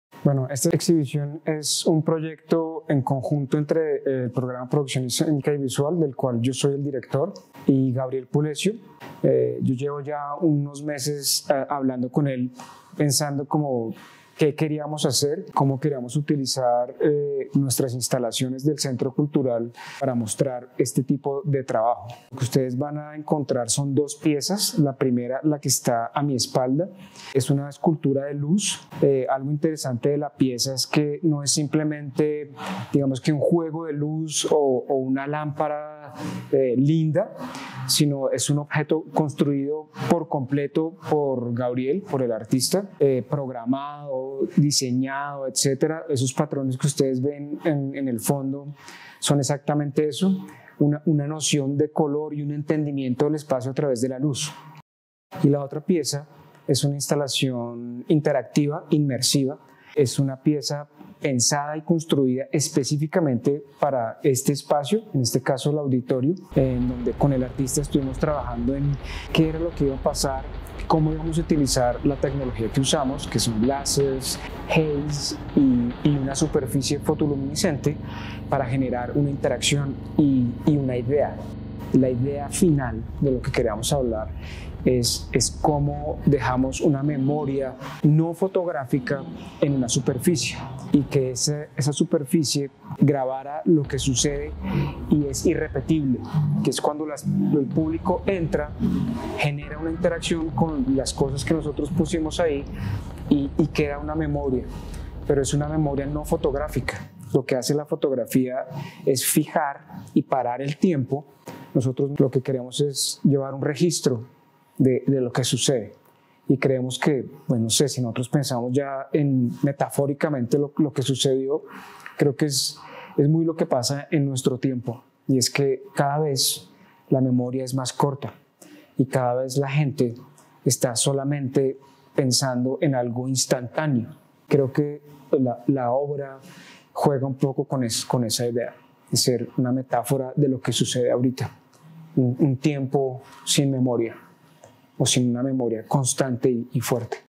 445NM PHOTPHANTOM · Producción Escénica y Visual
Daniel Mora, director del programa de Producción escénica y visual, nos cuenta de qué se trató esta exhibición y cuál fue la experiencia que creó Gabriel Pulecio @remembervictoriosa a través de dos piezas de arte inmersivo que tuvieron lugar en la sede cultural de LCI el pasado 10, 11 y 12 de Diciembre.